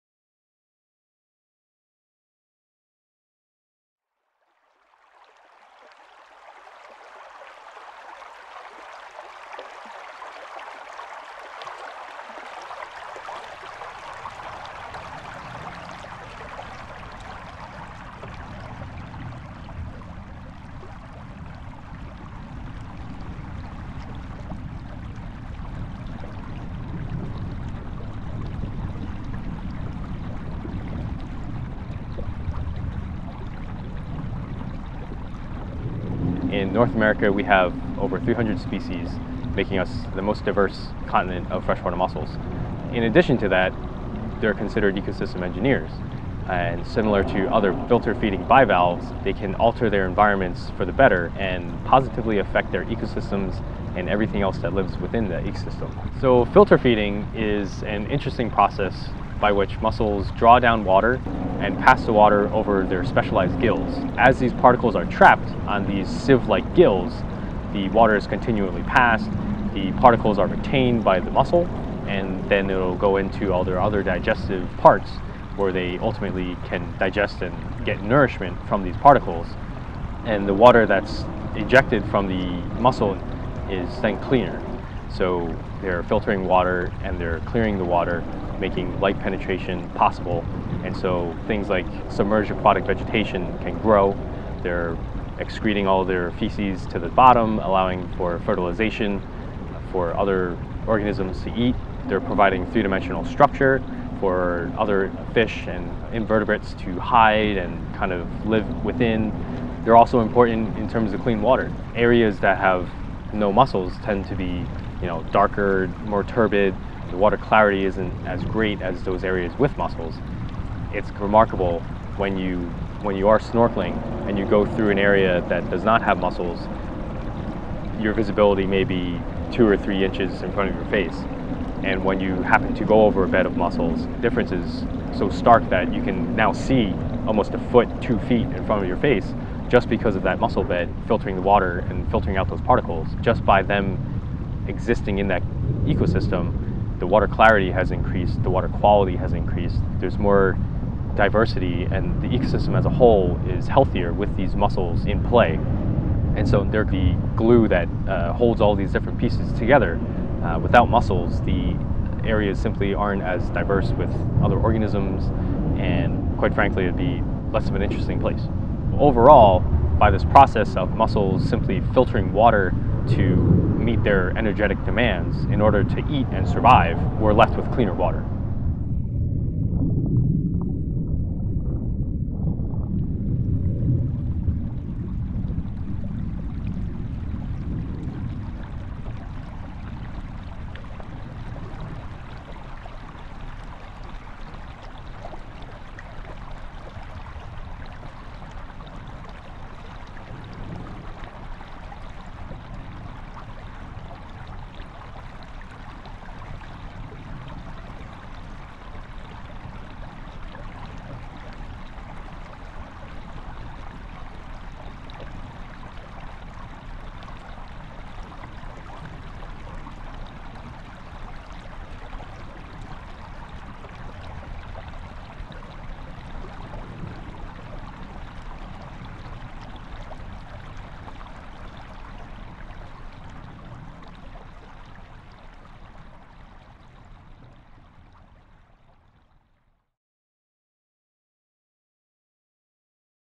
Mussels at Work: A Time Lapse Demonstration
Freshwater mussels make water cleaner, but don't take our word for it. See for yourself in this side-by-side, time-lapse demonstration.

This video was created for "The Mussel Hatchery" exhibit at the Fairmount Water Works. Read more about this on our website at ow.ly/vcZZ3096g60.

Video credit: Lendl Tellington of Pigeonbread, courtesy of Victoria Prizzia at Habithéque, Inc.